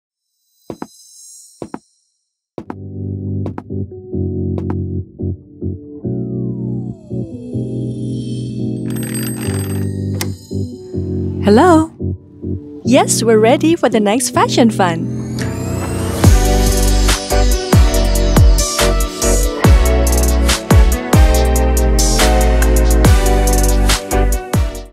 Barbie™ ❤️ Nada Puspita : The Vacanza Collection Teaser
The trails of fun haven’t ended yet🥳 They linger in our minds, fueling our zest to present them to you and filling you with excitement to embrace the days ahead with joy again💕

In collaboration with a stylish global icon. Are you ready to Shine On once more?✨